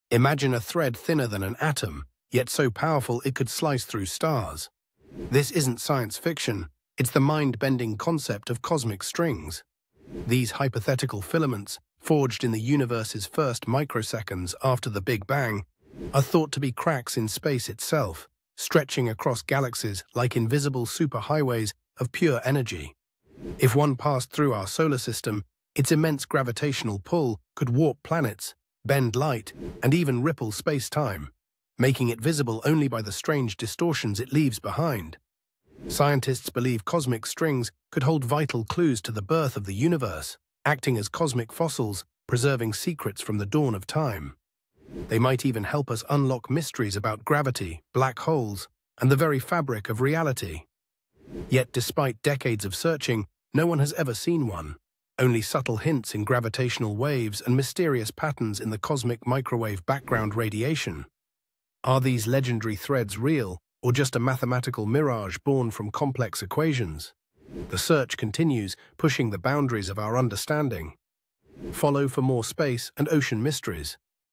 Cosmic Strings: Universe’s Hidden Superhighways
What if the universe had invisible superhighways of energy? Discover the mind-bending mystery of cosmic strings!